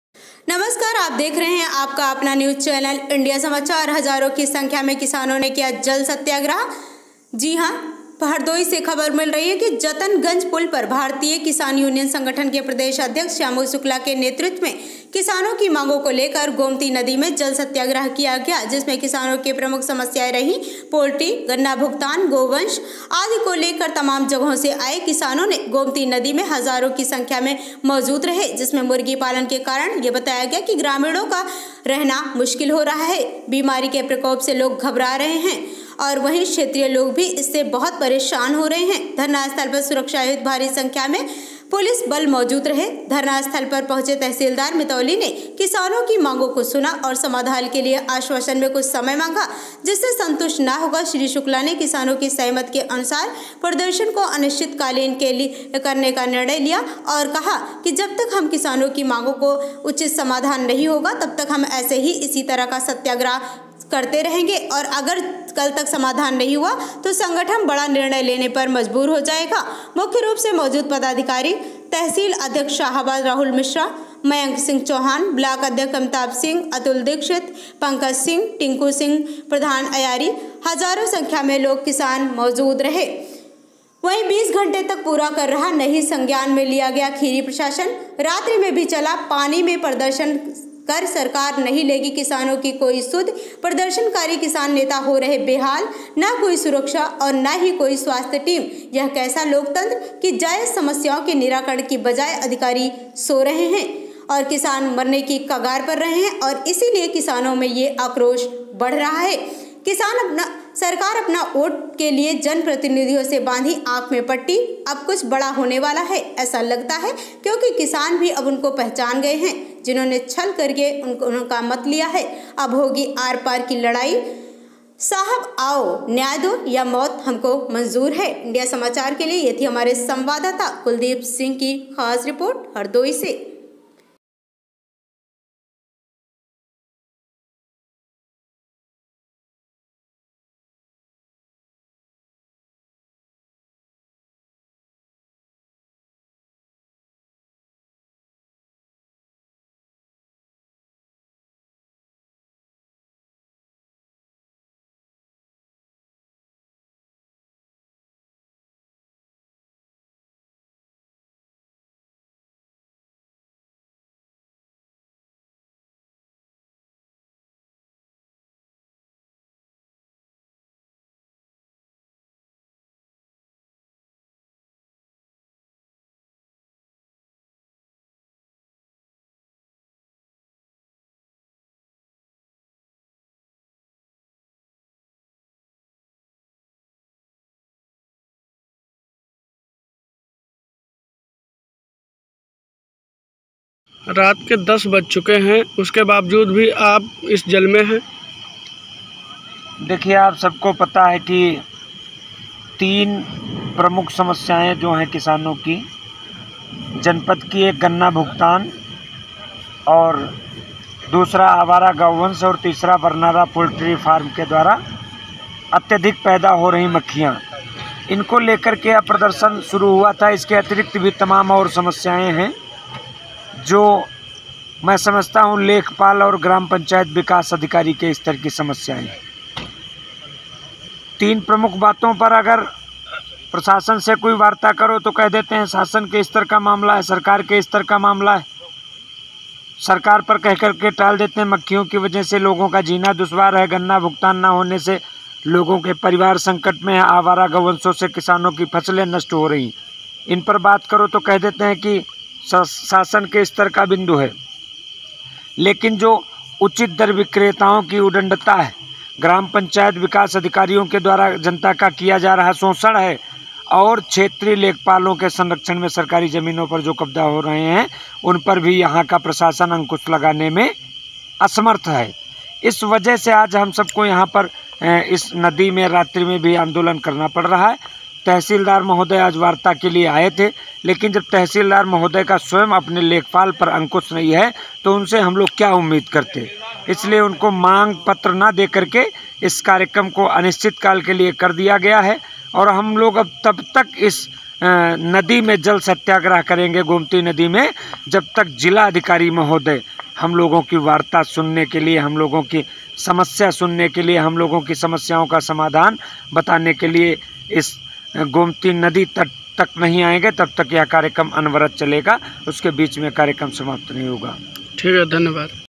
हजारों की संख्या में किसानों ने किया जल सत्याग्रह*। जतन गंज पुल पर भारतीय किसान यूनियन संगठ
पिहानी/हरदोई                            संवाददाता _कुलदीप सिंह।     .                           ***                                   *हजारों की संख्या में किसानों ने किया जल सत्याग्रह*।          जतन गंज पुल पर भारतीय किसान यूनियन संगठन के प्रदेश अध्यक्ष श्यामू शुक्ला के नेतृत्व में किसानों की मांगों को लेकर गोमती नदी में जल सत्याग्रह किया गया जिसमें किसानों की प्रमुख समस्याएं रही पोल्ट्री, गन्ना भुगतान ,गोवंश, आदि को लेकर तमाम जगहों से आए किसानों ने गोमती नदी में हजारों की संख्या में मौजूद रहे । जिसमे मुर्गी पालन के कारण ग्रामीणों का रहना हुआ मुश्किल ।बीमारी के प्रकोप से घबरा रहे ग्रामीण व क्षेत्रीय लोग । धरनास्थल पर सुरक्षा हेतु भारी मात्रा में पुलिस बल मौजूद । धरनास्थल पर पहुंचे तहसीलदार मितौली ने किसानों की मांगो को सुना समाधान के लिए  आश्वासन में कुछ समय मांगा ।जिससे संतुष्ट ना होकर श्री शुक्ला जी ने किसानों की सहमति के अनुसार प्रदर्शन को अनिश्चित कालीन करने का निर्णय लिया और कहा कि जब तक हम किसानों की मांगो का समाधान उचित नहीं होगा तब तक यह सत्याग्रह इसी तरह चलता रहेगा । और अगर कल तक समाधान नहीं हुआ तो संघठन बड़ा निर्णय लेने पर मजबूर होगा । मुख्य रूप से मौजूद पदाधिकारी  तहसील अध्य्क्ष शाहाबाद राहुल मिश्रा ,मयंक सिंह चौहान, ब्लॉक अध्यक्ष अमिताभ सिंह , अतुल दीक्षित , पंकज सिंह , टिंकू सिंह प्रधान अयारी,   समेत हजारों की संख्या में किसान मौजूद रहे ।                   पूरी रात चला सत्याग्रह। --

*✒️ किसान नेताओं का चल रहा जल सत्याग्रह लगभग 20 घण्टे पूरे  कर रहा नही संज्ञान ले रहा खीरी प्रसासन रात्रि में भी चला  पानी मे प्रदर्शन  सरकार नही लेगी किसानों की सुध प्रदर्शन कारी किसान नेता हो रहे बेहाल न कोई सुरक्षा न कोई स्वास्थ्य टीम यह कैसा लोकतंत्र की जायज समस्यओं के निराकरण के बजाय अधिकारी सो रहे  । किसान मरने की कगार पर*किसानों में बढ़ रहा आक्रोश
किसान नेता श्यामू शुक्ला ने कहा --
✒️सरकार सपना पूरा वोट लिए जन प्रतिनिधियों ने भी बांधी आंख में पट्टी अब कुछ बड़ा होने वाला है ऐसा लगता है क्योंकि किसान भी अब उनको पहचान गए है जिन्हों ने छल करके उनका मत लिया है *अब होगी आर पार की लड़ाई साहब आओ न्याय दो या मौत हमको मंजूर है।*